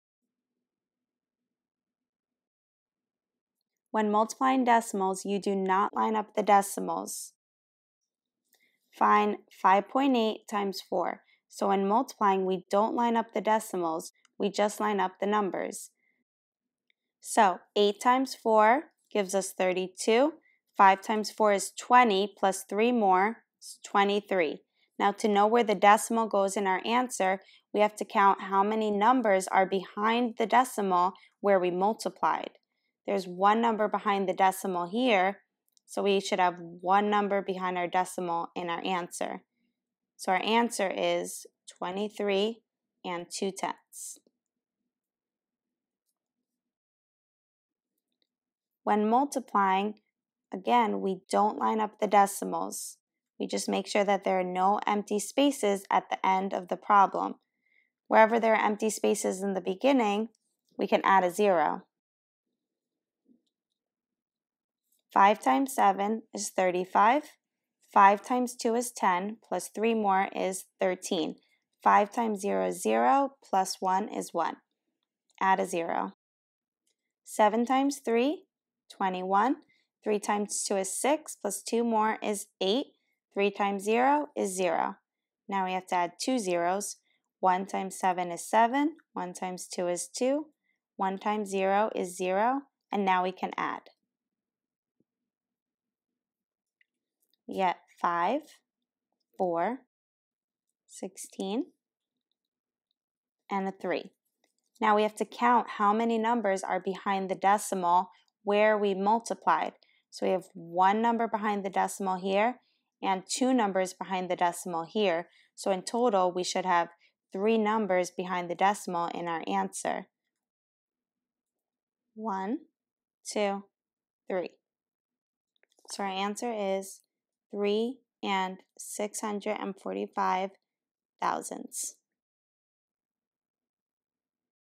Multiplying Decimals
This is a how to math tutorial video on multiplying decimals.